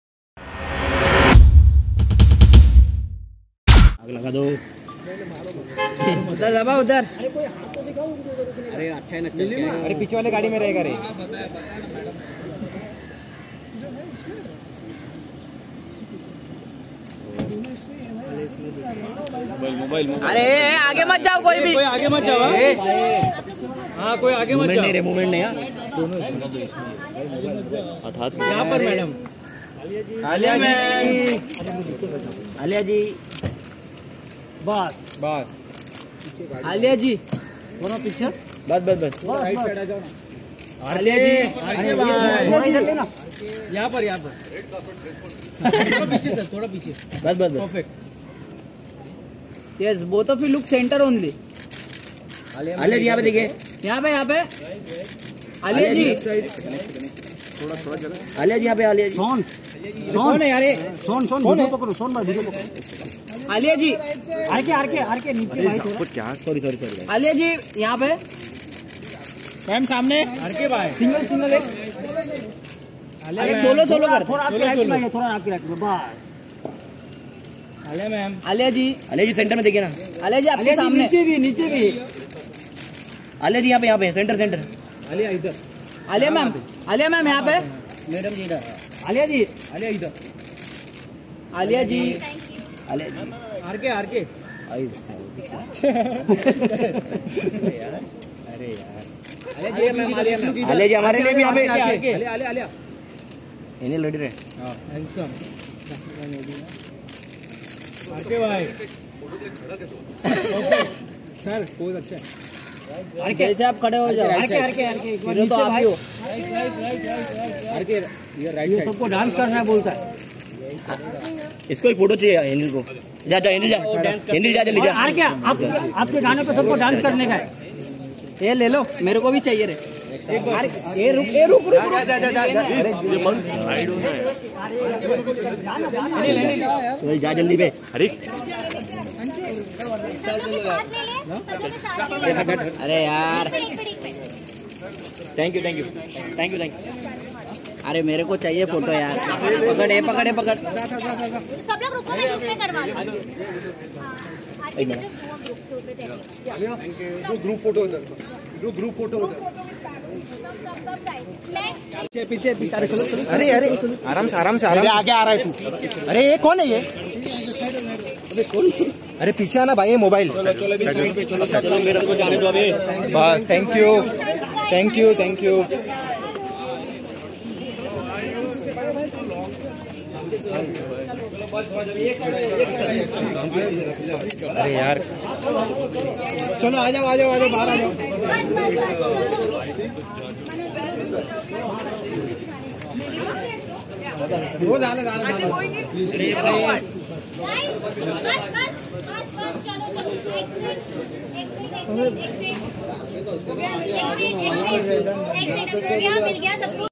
Ranbir Kapoor And Alia Bhatt Spotted At Mehboob Studio Fro Bhramastra Promotion
ranbirkapoor And aliabhatt Spotted At Mehboob Studio Fro Bhramastra Promotion